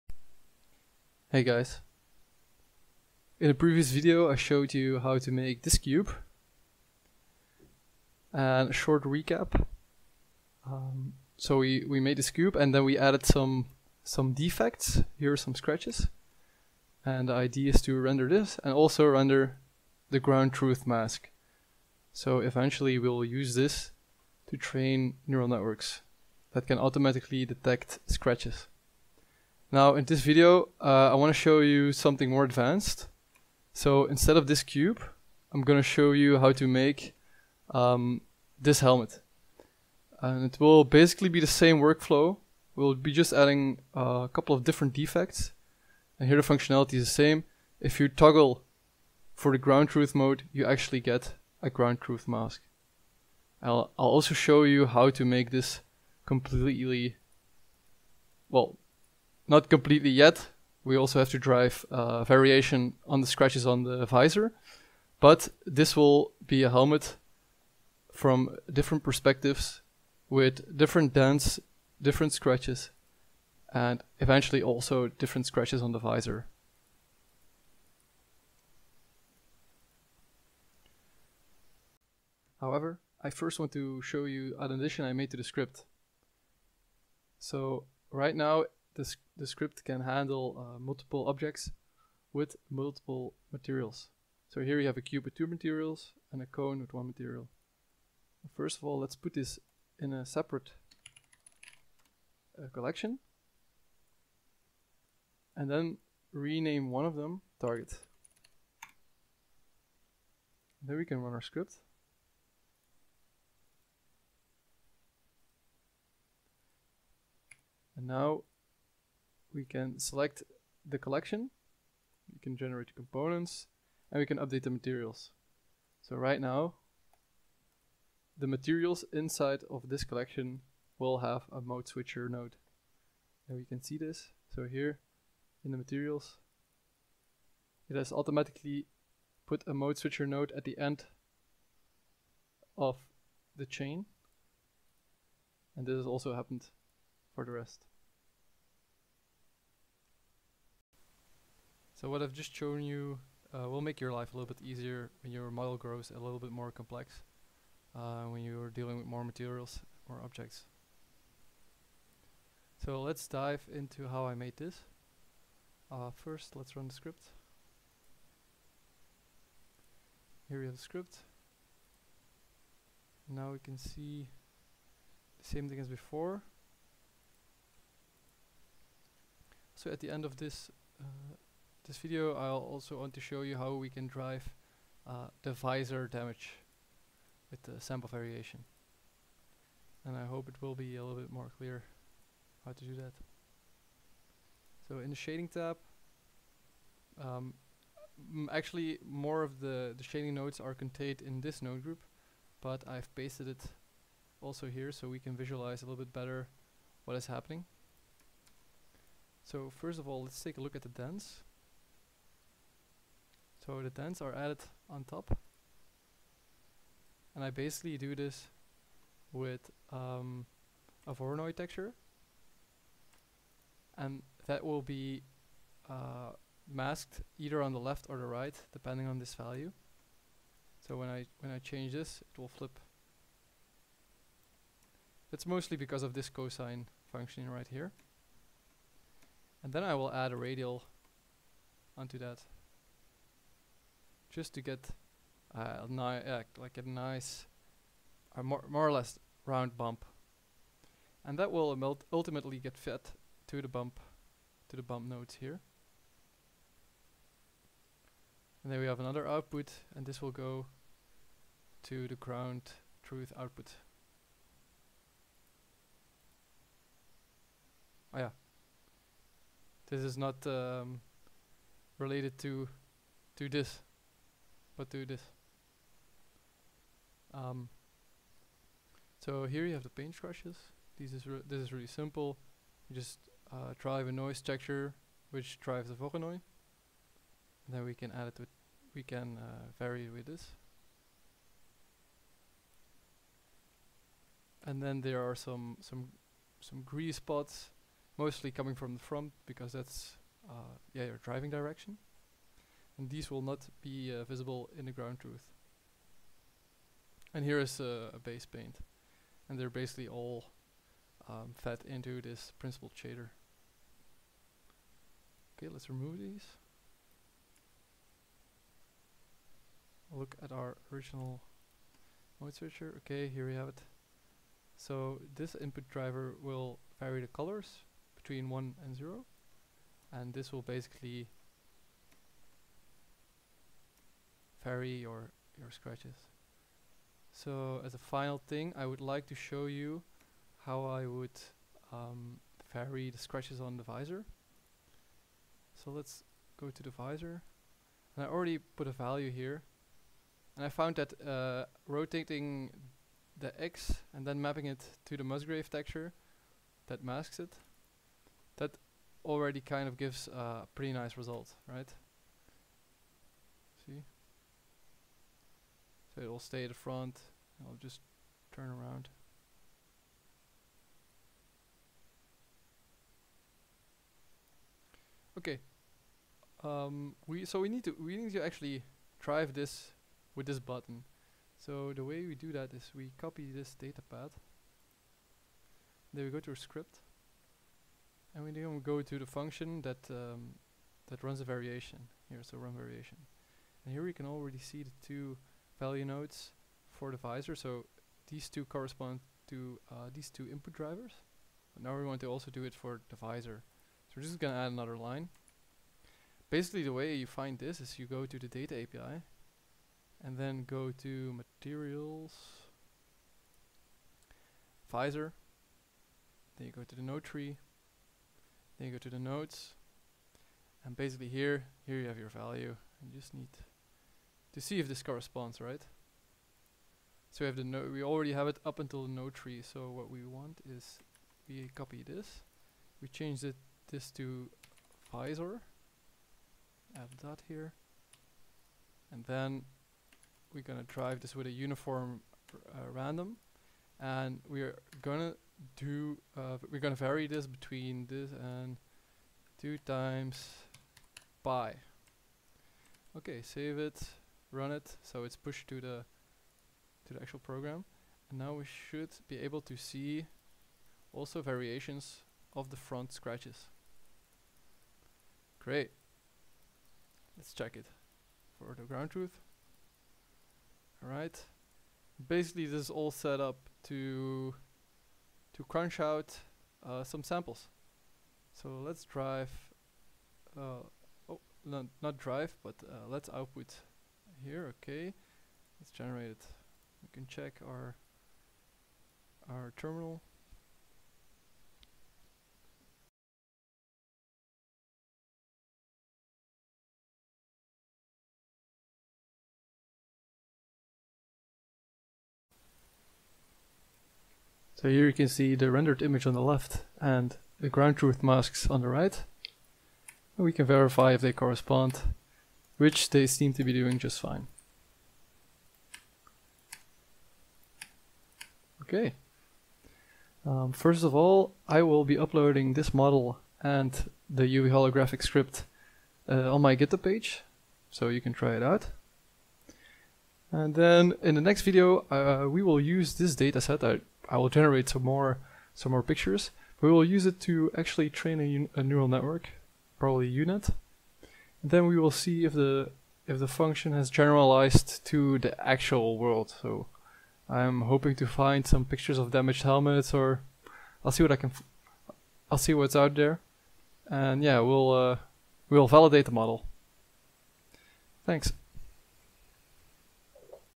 Generating Machine Learning Data in Blender: part 2 - Advanced Defects
This video shows a small addition to the uvHolographics script and showcases a more complex model. The data can be used for semantic segmentation.